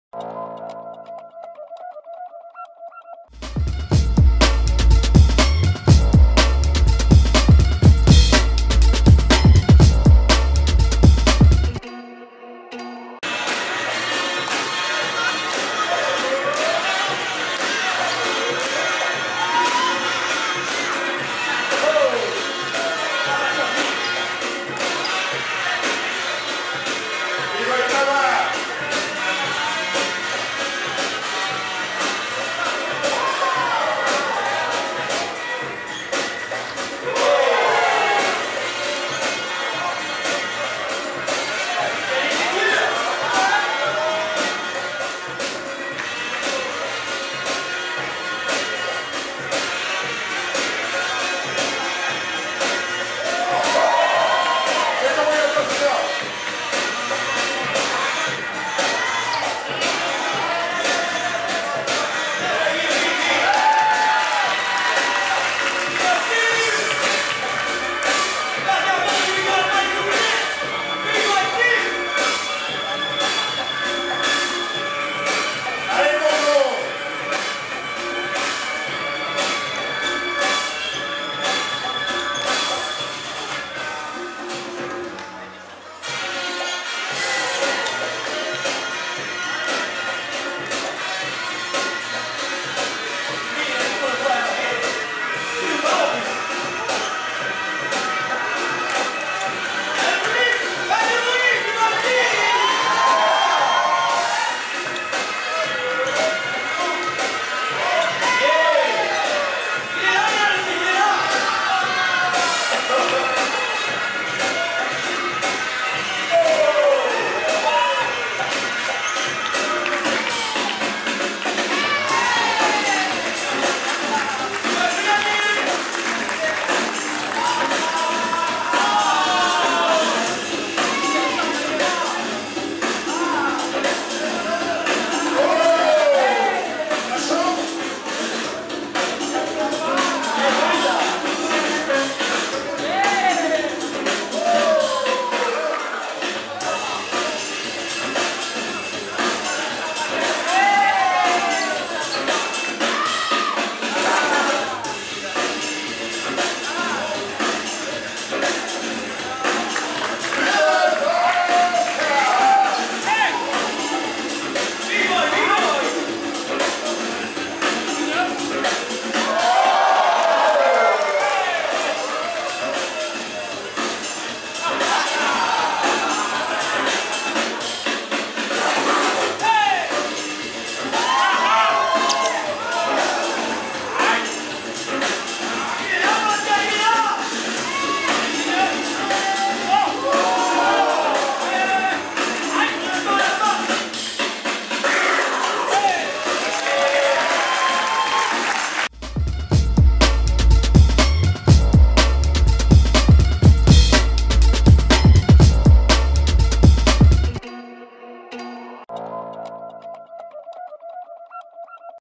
Vagabond vs WBB | Top16 | Freestyle Session France 2018
Freestyle session france produced by 9.2 Styles, Powered by udef and silverback events. day 3 of the Breakleague, Frances first competitive bboy league composed of 16 events throughout the year all linked by a ranking system. Top 3 bboys of the league fly out to silverback open 2019.

Winners:
WBB